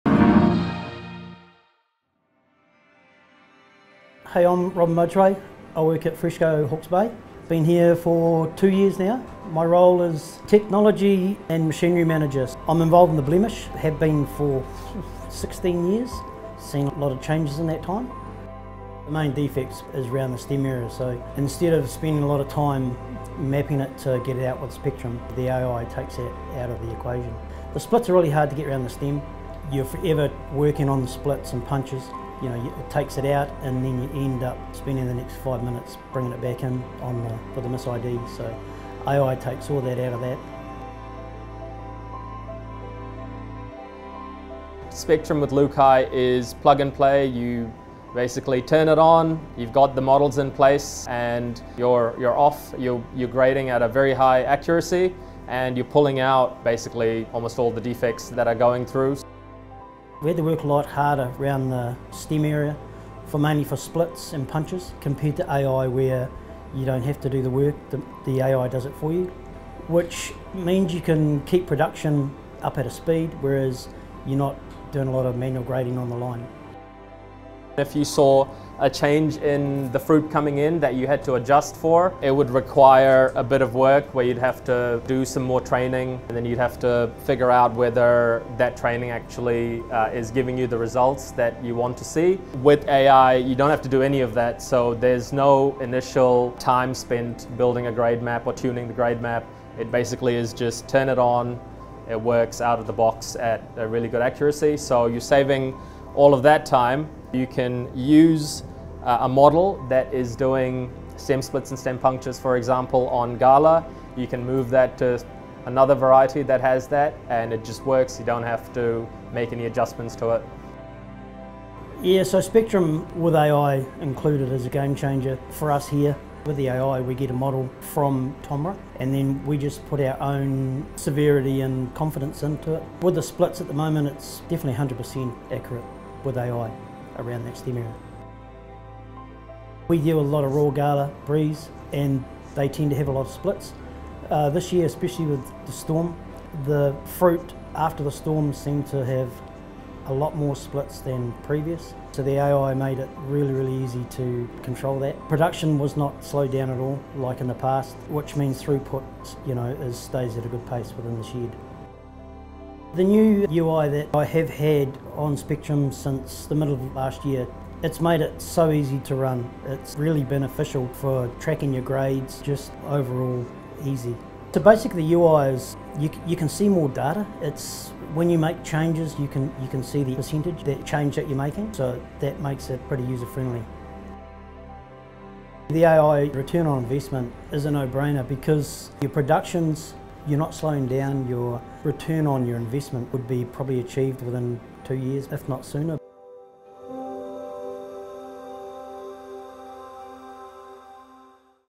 Customer Story - Freshco - Spectrim with LUCAi
Freshco, a leading New Zealand fruit and vegetable grower and exporter, shares the benefits they've experienced with the implementation of the Spectrim LUCAi™ upgrade package on their apple line.

Spectrim with LUCAi™ includes new ‘plug-and-play’ Deep Learning models, pre-trained by vast datasets, that can precisely detect and classify even the trickiest apple defects, such as splits and punctures across multiple varieties. Spectrim with LUCAi™ allows customers to easily adjust the severity of grading parameters – something which previously had to be done by experienced operators – to cater for seasonal dynamics and market preferences. And the pre-trained intelligence of this upgrade is customizable.